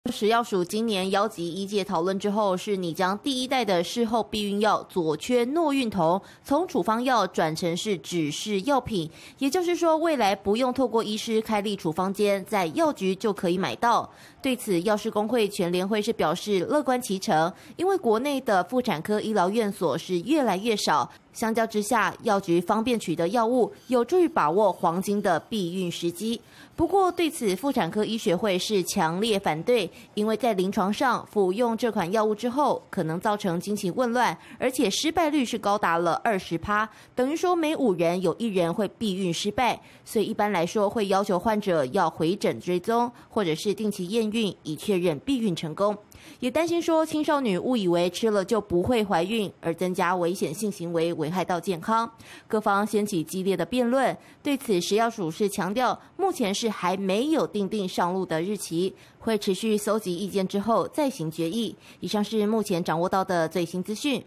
事後丸擬改為指示藥 藥局就能買掀激辯－民視新聞
【民視即時新聞】民視新聞／綜合報導為了避免非預期懷孕，不少女性會服用事後丸，食藥署擬將第一代事後避孕藥「左炔諾孕酮」，改為指示藥品，讓民眾在藥局就能購買，詳情連線記者鄭筑勻。